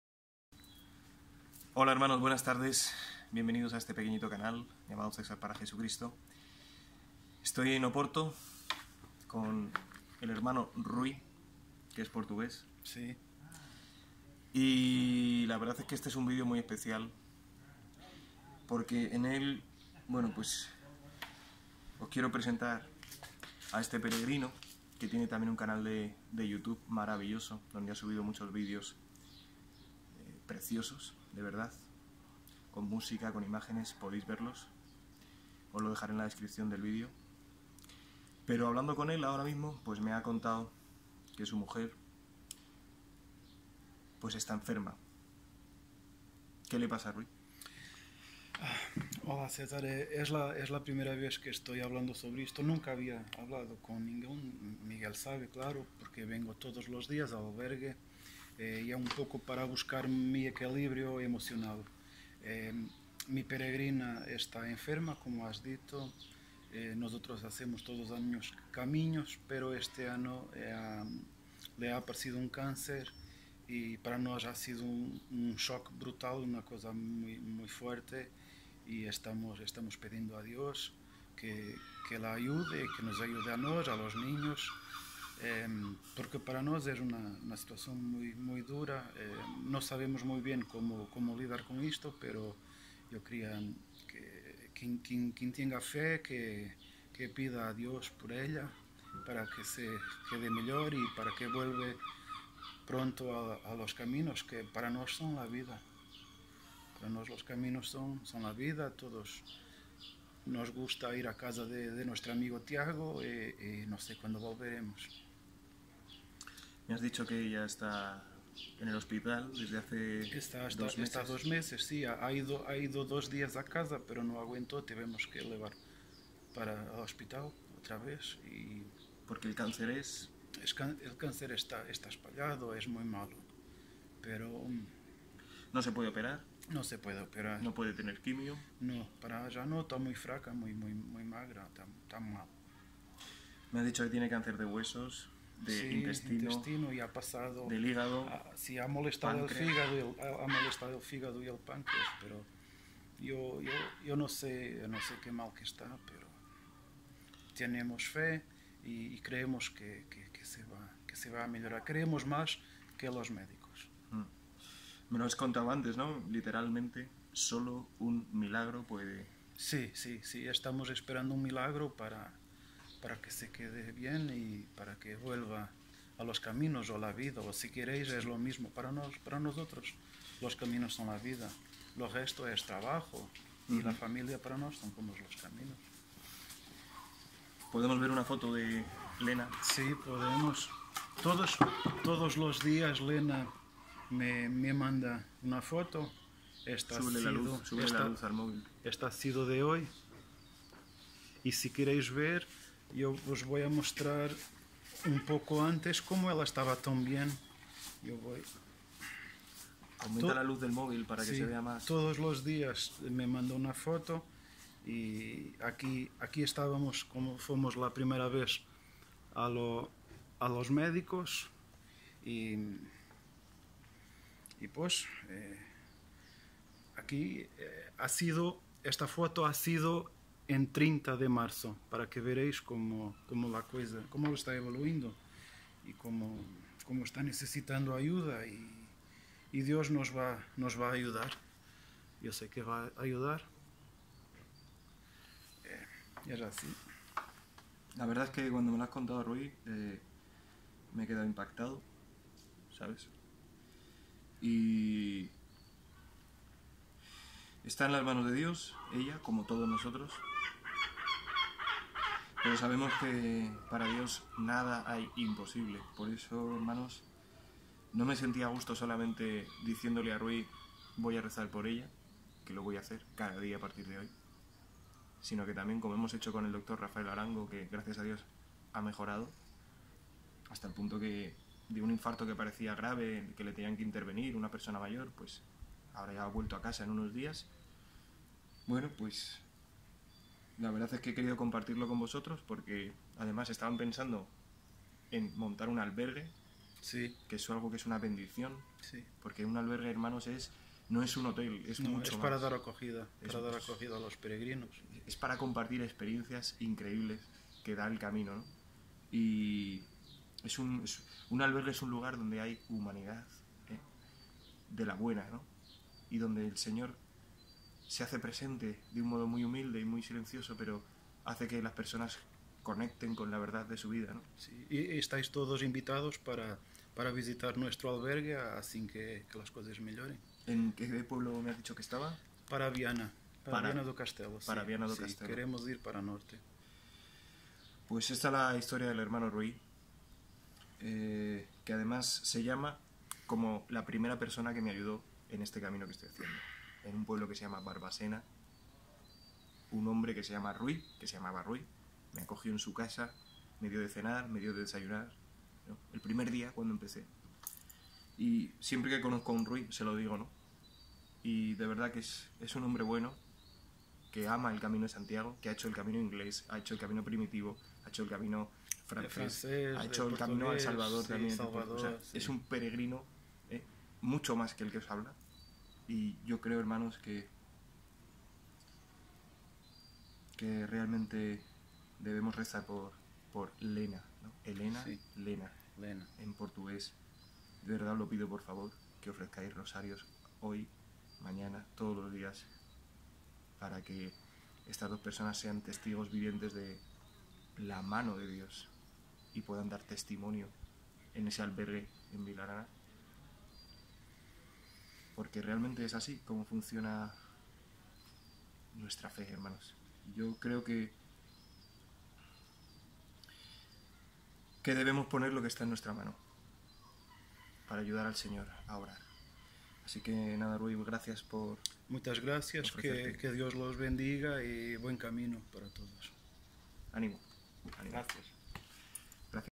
🙏Esperamos un MILAGRO: ¡Ayuda, hermanos!
Por favor, mirad este vídeo que hicieron juntos, Rui y Lena. En aquella peregrinación él le regalaba a ella una flor distinta cada mañana. ¿No es eso verdadero amor?

Por favor, os lo pido, orad por ellos.

En un hombre fantástico, que me ha ayudado mucho, así como el resto de responsables de este albergue de Oporto.

Os dejo este otro vídeo que me ha impactado al verlo, en él podréis sentiros auténticos peregrinos:

Dios os lo pague, os bendiga, y la Virgen os guarde de todo mal hoy y po siempre. Amén +++